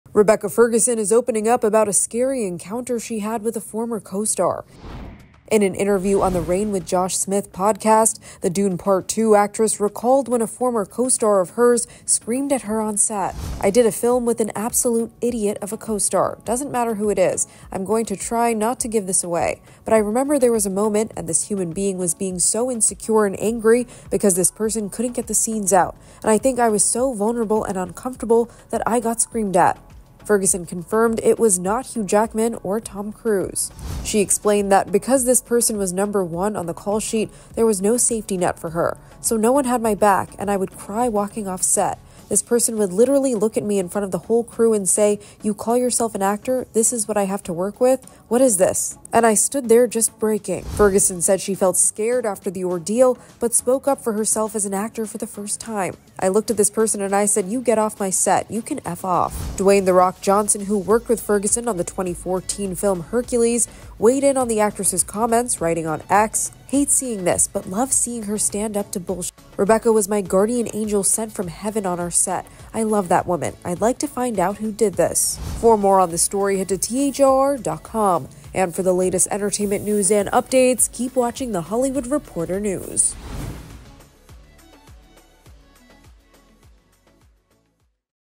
Rebecca Ferguson Recalls Getting Screamed at by "Idiot" Co-Star on Set | THR News
Rebecca Ferguson is opening up about a scary encounter she had with a former co-star. In an interview on the 'Reign With Josh Smith' podcast, the 'Dune: Part Two' actress recalled when a former co-star of hers screamed at her on set. "I did a film with an absolute idiot of a co-star — doesn't matter who it is. I'm going to try not to give this away, but I remember there was a moment, and this human being was being so insecure and angry because this person couldn't get the scenes out. And I think I was so vulnerable and uncomfortable that I got screamed at."